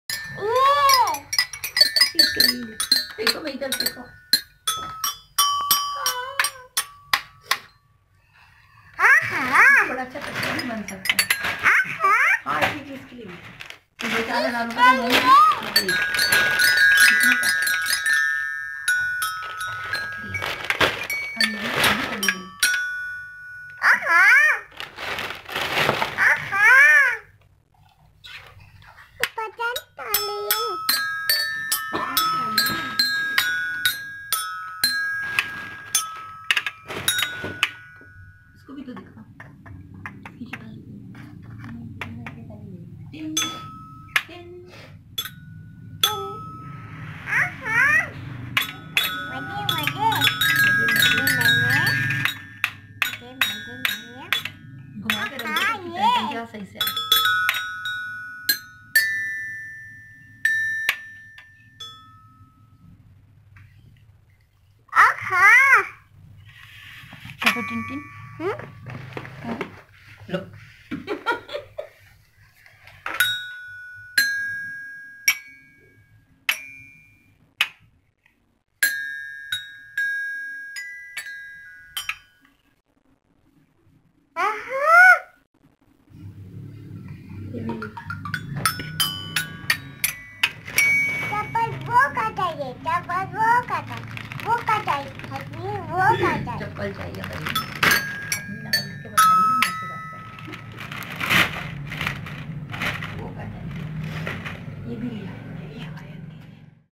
Funny Kid Playing First time with Xylophone | Seems Very Happy to Play
FunnyKid Playing First time with Xylophone | Seems Very Happy to Play

This type of toys are very cool for HomeSchooling, and helpful for child to learn alot, look how she is reacting when playing xylophone for the very first time.